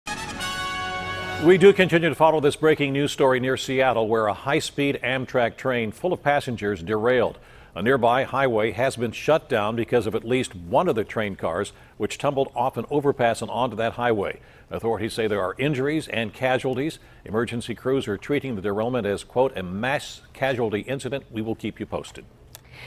Amtrak Train Derails In Washington State
Officials are on the scene of a reported mass casualty event involving an Amtrak derailment in Washington State.